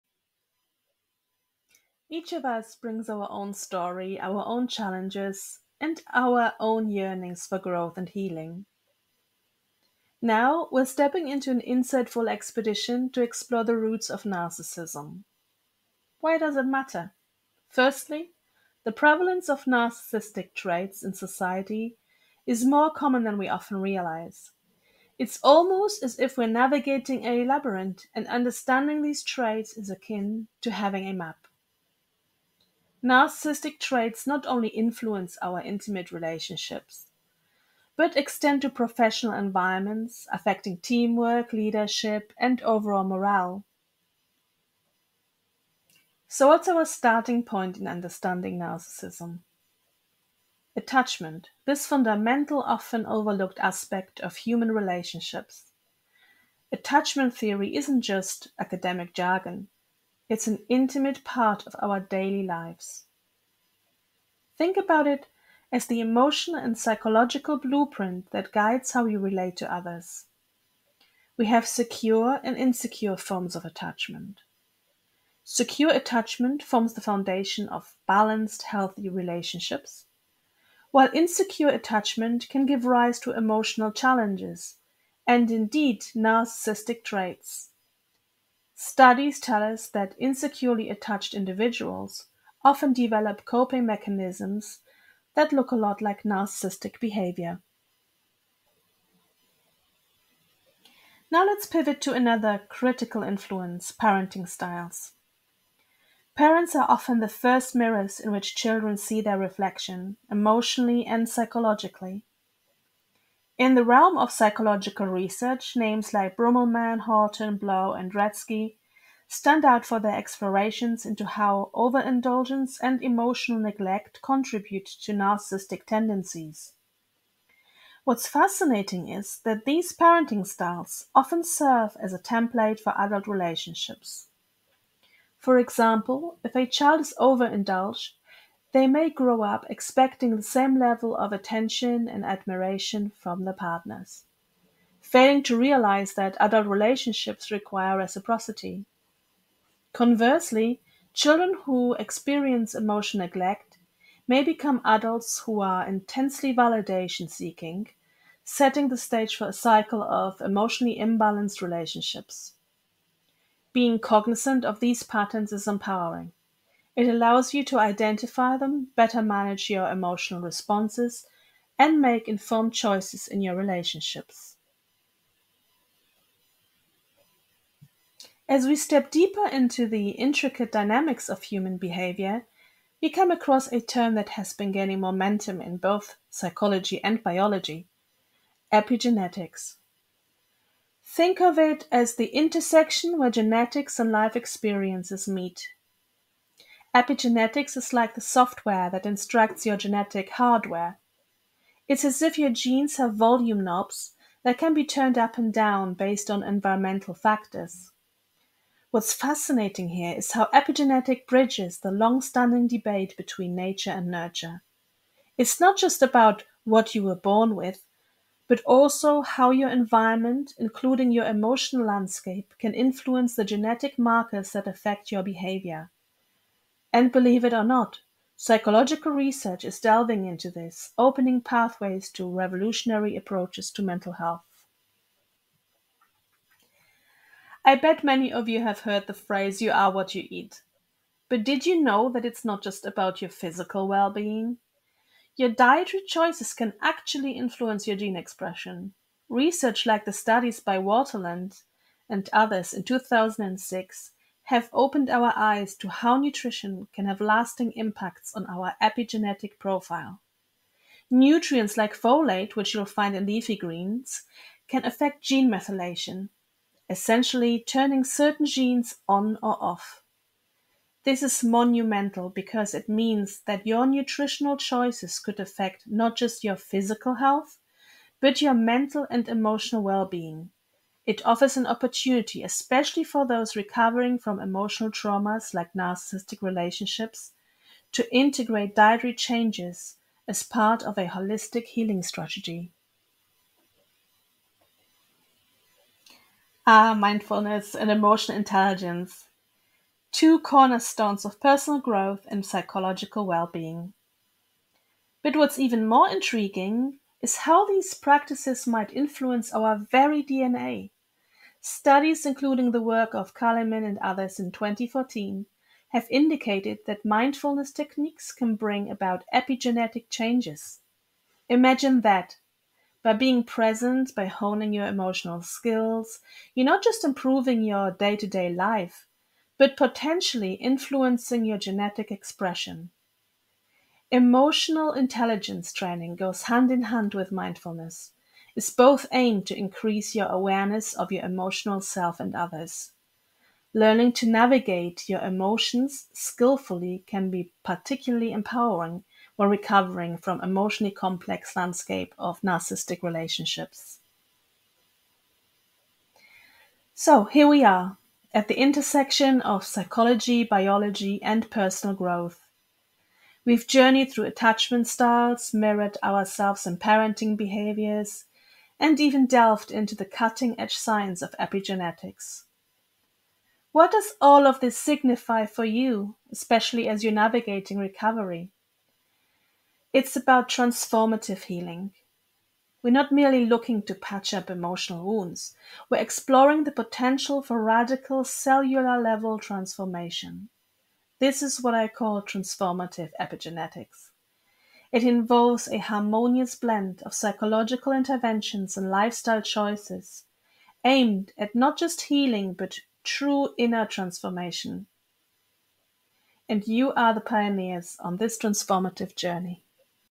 The Roots of Narcissism
Where does Narcissism come from? Are narcissistic individuals born or made? Is narcissism nature or nurture? Or both?
This video is part of our 10-Week Recovery from Narcissistic Relationships Program.  The first week is free.